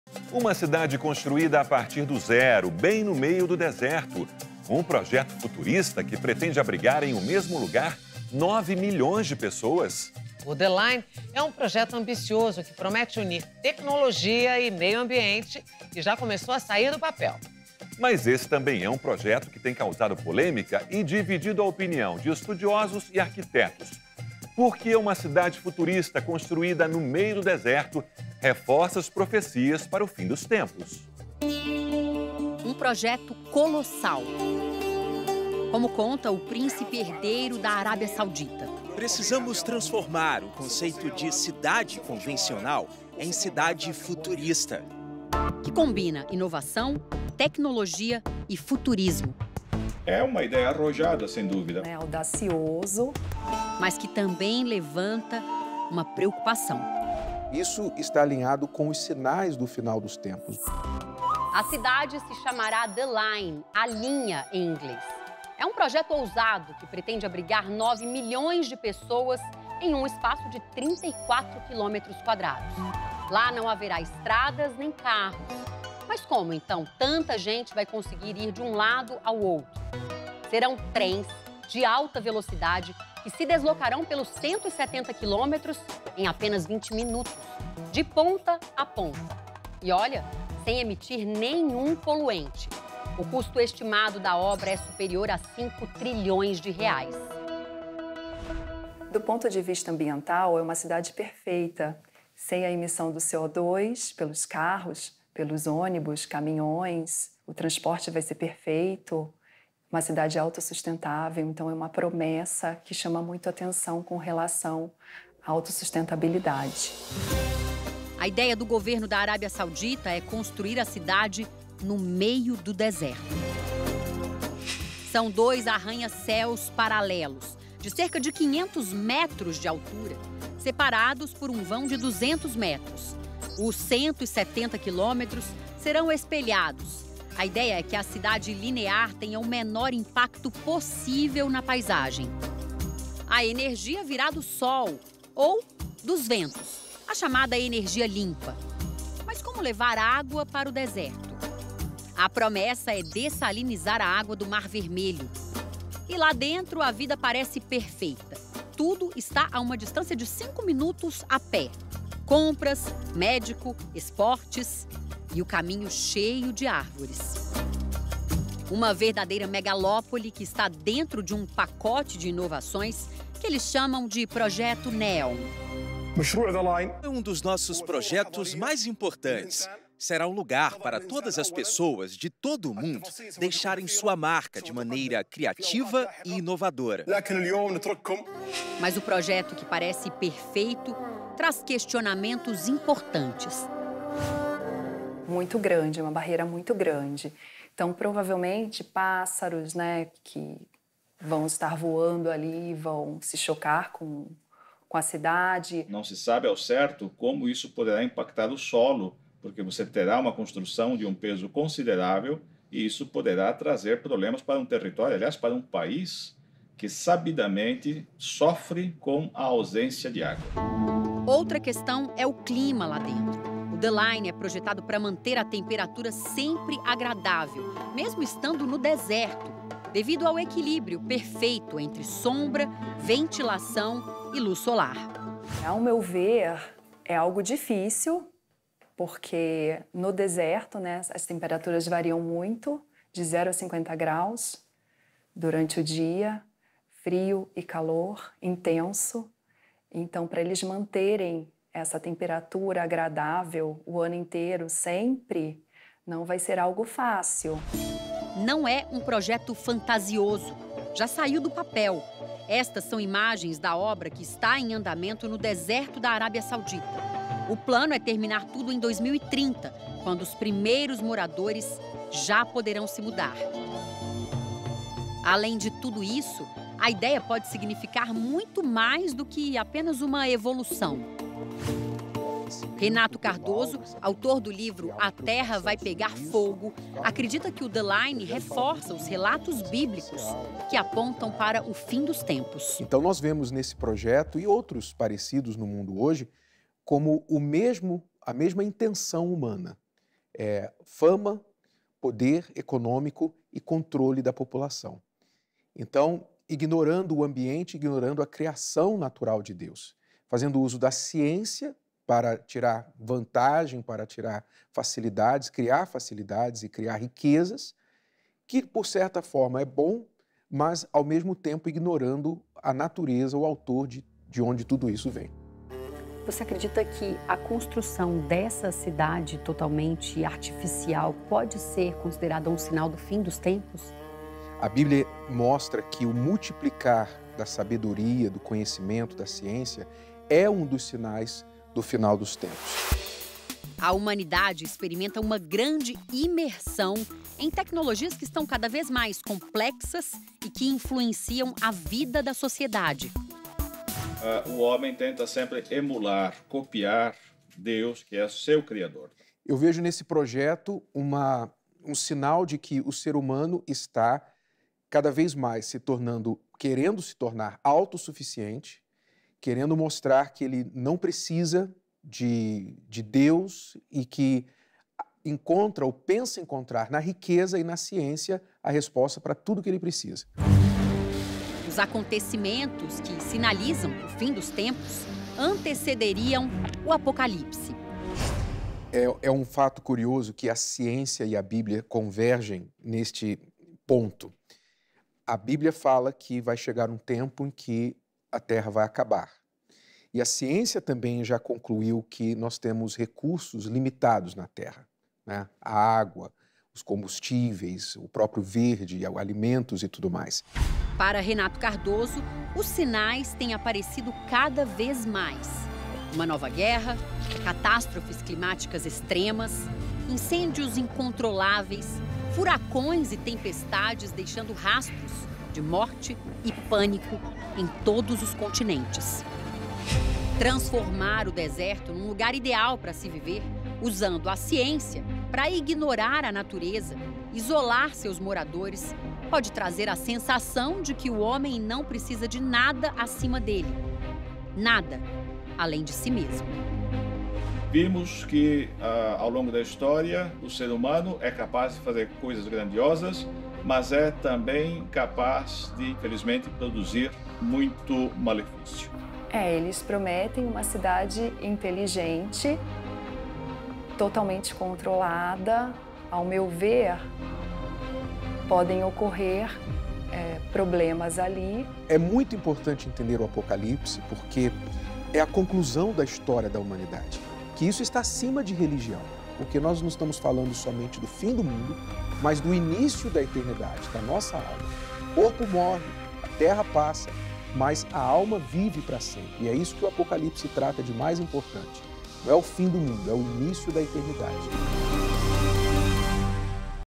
Obra futurista ‘The Line’ promete unir tecnologia e meio ambiente no deserto
Uma cidade construída a partir do zero, bem no meio do deserto. A obra futurista pretende abrigar, em um mesmo lugar, nove milhões de pessoas O ‘The Line’ é um projeto ambicioso que promete unir tecnologia e meio ambiente. O custo estimado do projeto passa do equivalente a R$ 5 trilhões. Essa iniciativa tem causado polêmica e dividido a opinião de estudiosos e arquitetos. Entenda!